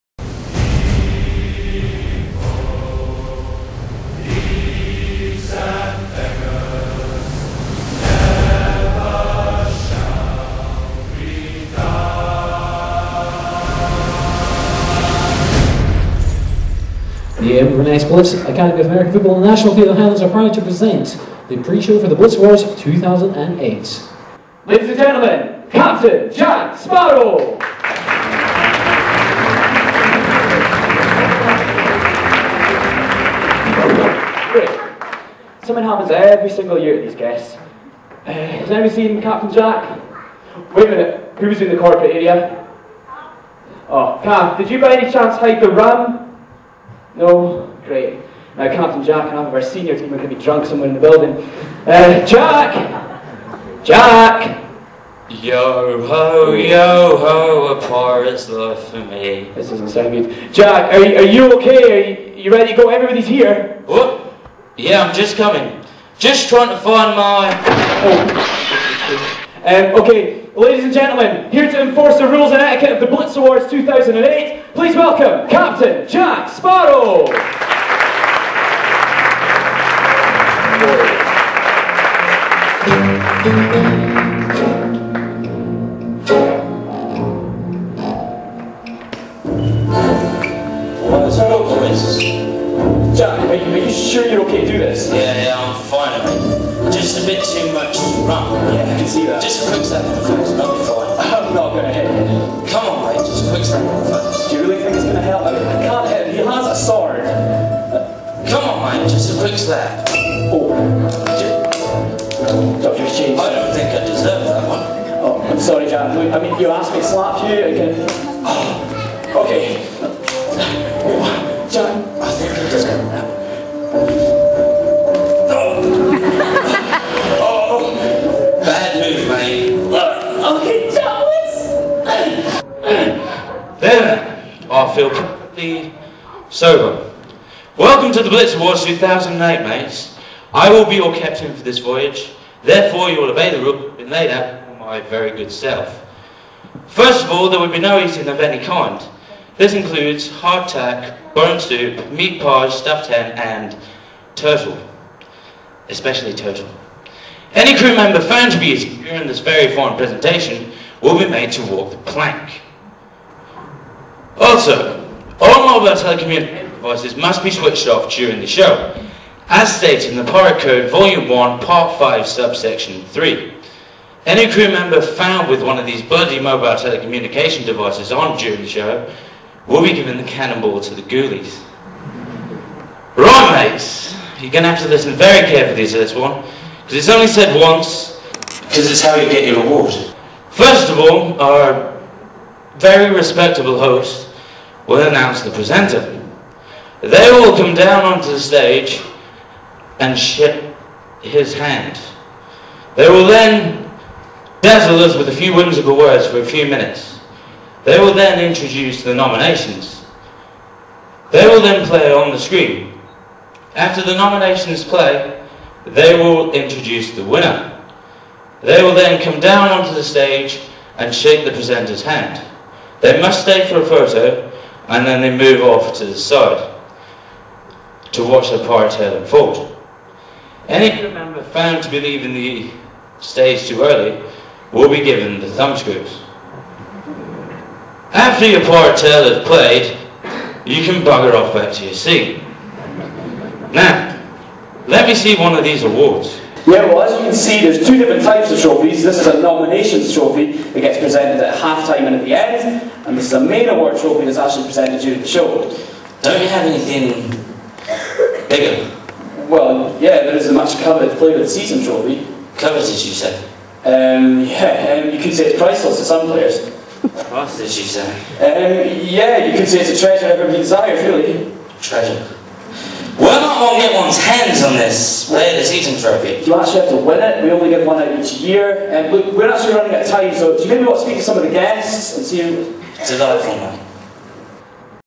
BLITZ AWARDS 2008 (Part 1) - Capt Jack
This was the opening to the BLITZ AWARDS 2008.  It saw Captain Jack Sparrow introduce the rules and ettique of the event...but he was after something else!